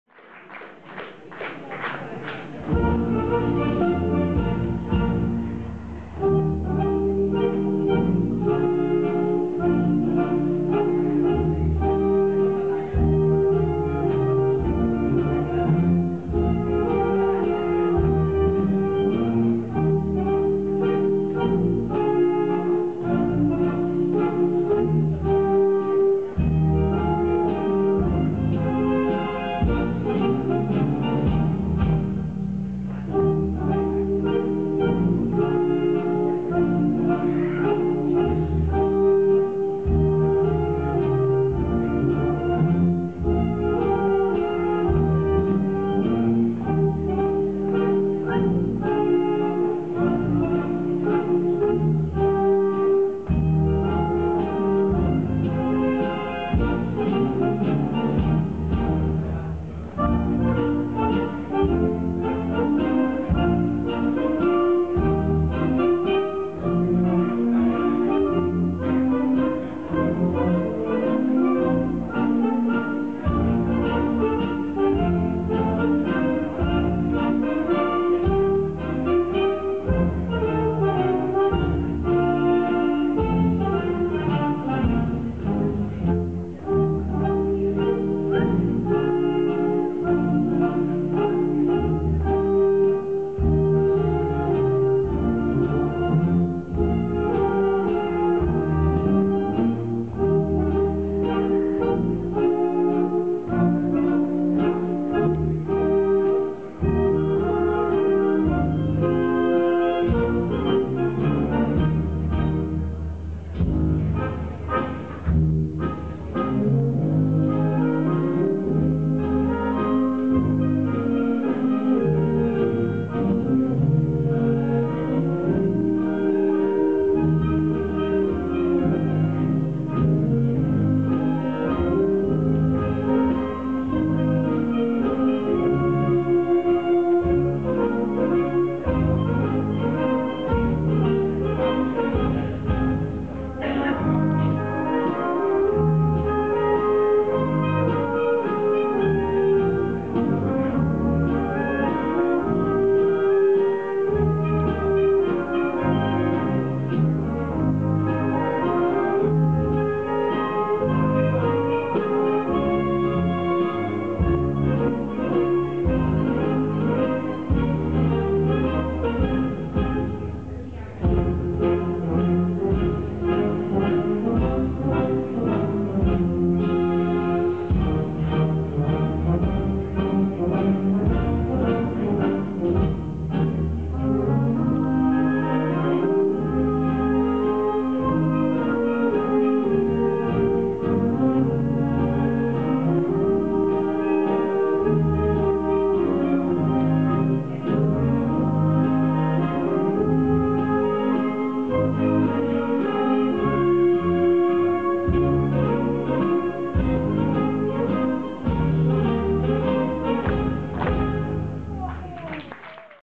TANZGRUPPE DES LUDWIG HOLLOS KUTURVEREIN KROTTENDORF MAZURKA
Eine ungarndeutsche Mazurka von der Amateurtanzgruppe Krottendorf getanzt. Beim Chor und Tanztreffen im Kulturhaus von Krottendorf/Bekasmegyer im III. Bezirk von Budapest